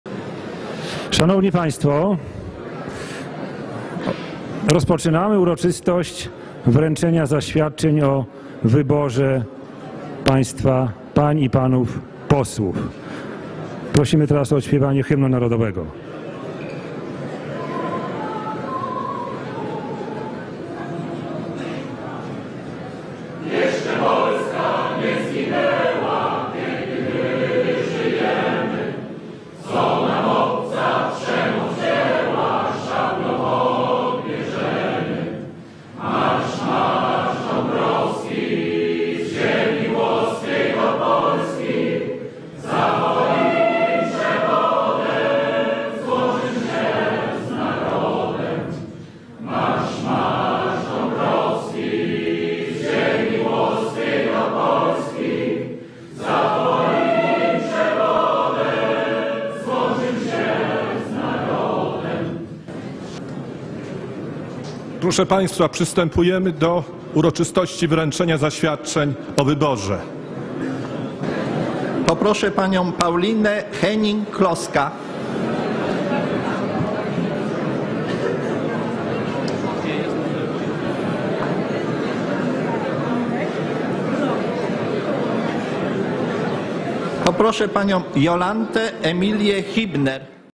Paulina Hennig Kloska - 4 listopada 2015 r. - wręczenie zaświadczenia o wyborze
Popierasz? Udostępnij! Paulina Hennig Kloska - 4 listopada 2015 r. - wręczenie zaświadczenia o wyborze

W środę, 4 listopada o godz. 12.00 w Sali Kolumnowej w Sejmie odbyła się uroczystość wręczenia posłom VIII kadencji zaświadczeń o wyborze, zorganizowana przez Państwową Komisję Wyborczą. Wręczenie zaświadczeń nowo wybranym posłom wynika z art. 239 ustawy z 5 stycznia 2011 r. Kodeks wyborczy.